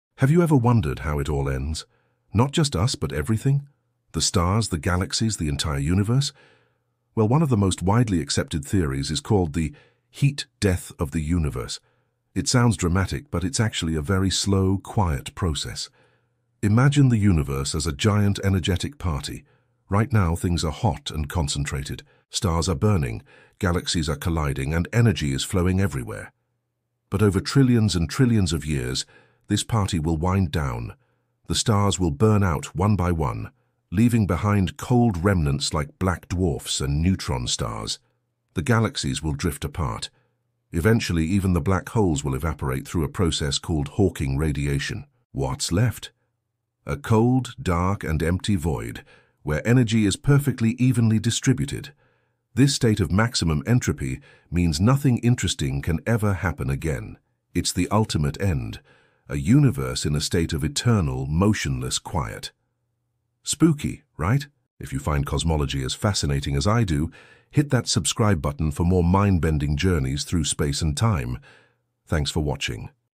The Heat Death: How the Universe Will End 🌌🕯️
When the last stars die, and even black holes fade, the universe will freeze into silence. This is how everything ends. 🌌🕯️